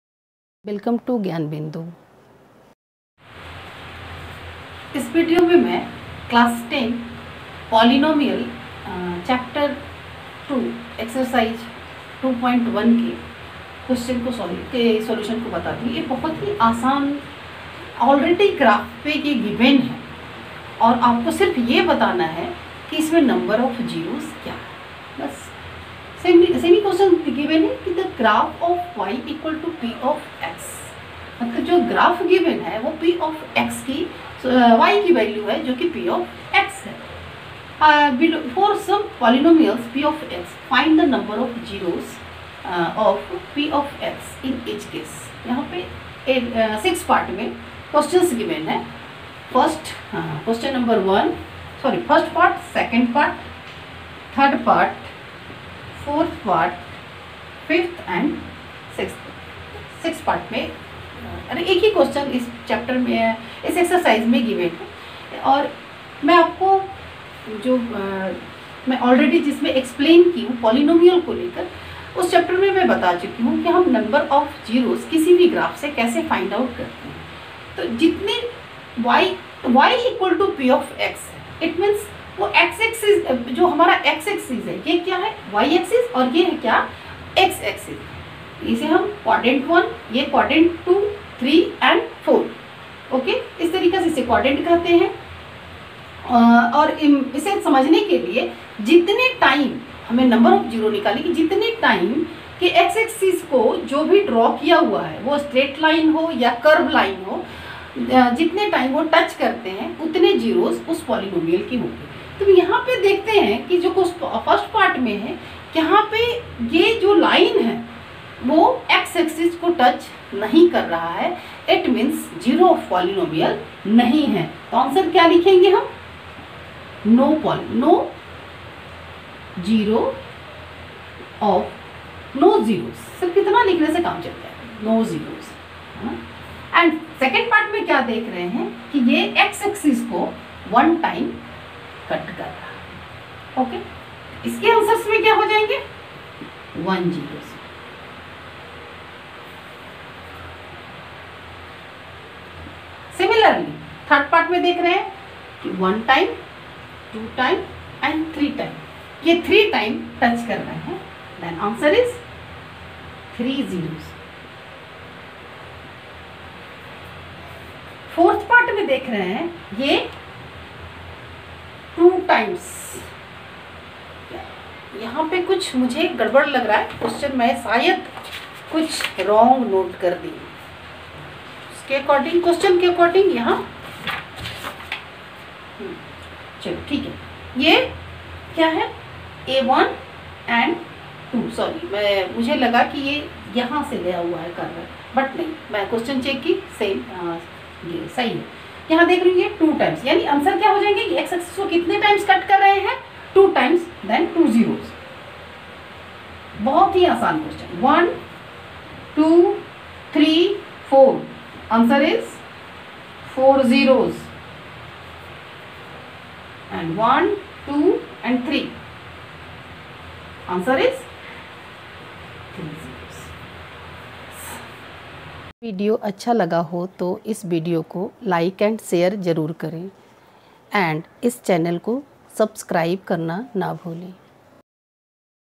Class 10 | Polynomials - Exercise 2.1 | Solved | NCERT | Math.
This video is about the solution of Polynomials of Class 10 Exercise 2.1.
All the questions are explained in very easy manner.
Hope this video will help you a lot before going for exams.